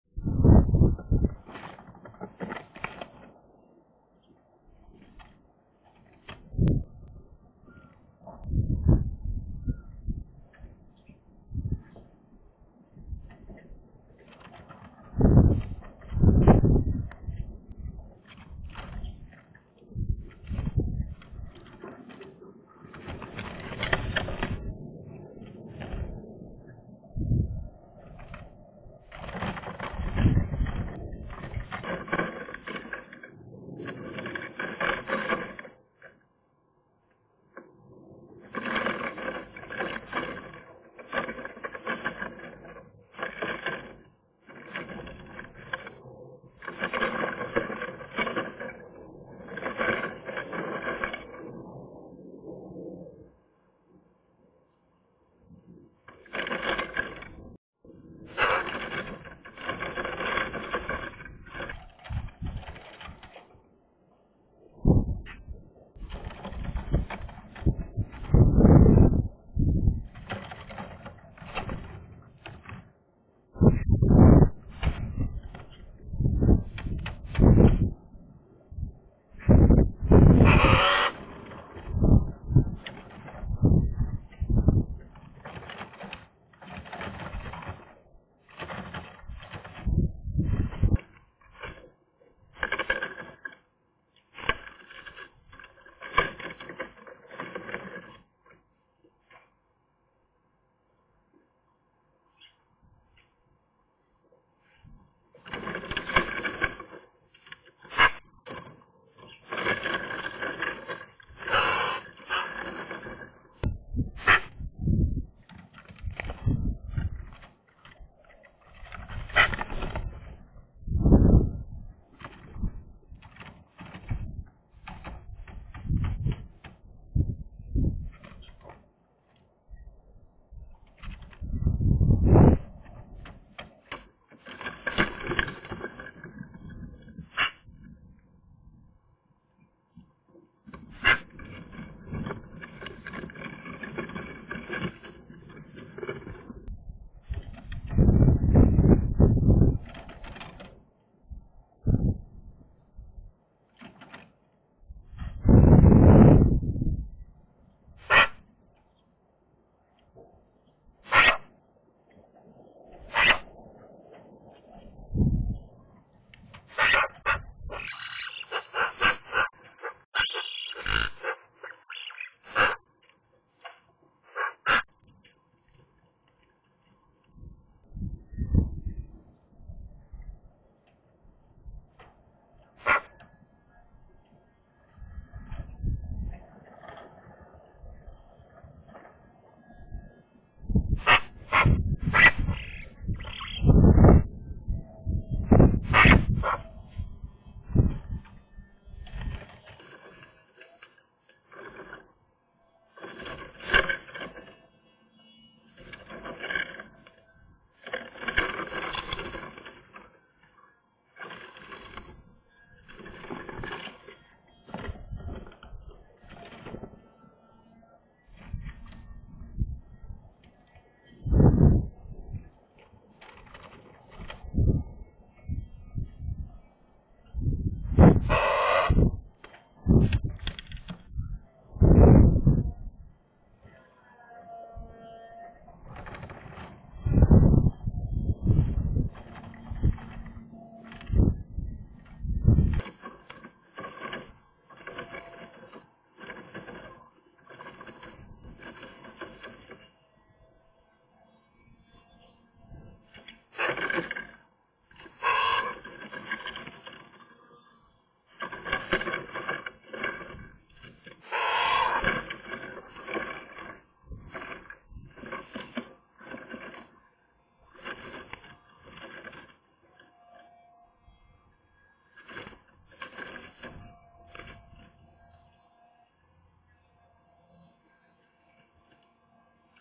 210327 magpie nest initial pit
The magpies are getting pretty satified with the walls in the nest.
Now the project is about to enter the next phase.
The first little load of pit material is brought by Hemag.
It is root threads mixed with mud.
Shemag is content.